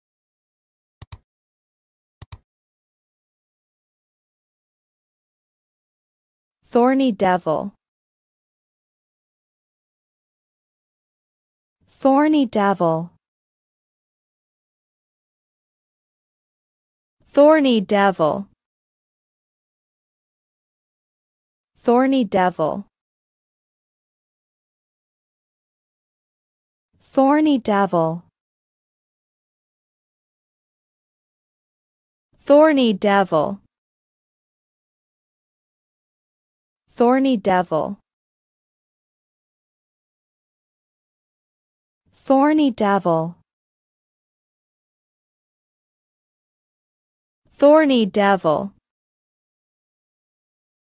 How to pronounce Thorny Devil in English correctly
How do you Say (Thorny Devil)? Learn and listen to how to Pronounce (Thorny Devil) in English!

The Thorny Devil, also known as the Moloch horridus, is a unique lizard species found in the arid regions of Australia. It is known for its distinctive spiky appearance, which provides camouflage and protection from predators.

Thorny Devils are small lizards, typically growing up to 20cm in length. Their skin is covered in small, thorn-like spikes, which help them blend in with the surrounding environment. Their coloration ranges from pale yellow to reddish-brown, depending on the color of the sand or rocks in their habitat.

One of the most notable features of the Thorny Devil is its ability to drink water through its skin. They collect water from dew or rain using their skin and then transport it to their mouths by capillary action. They can also drink from puddles using their tongues.

Thorny Devils primarily feed on ants, which they catch using their long, sticky tongue. They are also known to eat termites and other small insects. Despite their fearsome appearance, they are not dangerous to humans and are generally docile.

The Thorny Devil is a protected species in Australia, where it is considered to be vulnerable to extinction. Threats to their survival include habitat destruction, feral animals, and climate change.

00:00 Intro
00:07 How to Pronounce
00:16 Repeat more